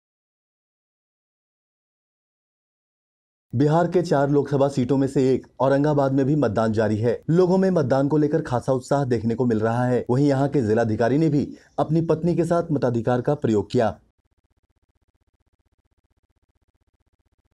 डीएम ने अपनी पत्नी के साथ डाला वोट
मतदान को लेकर मतदाताओं में खासा उत्साह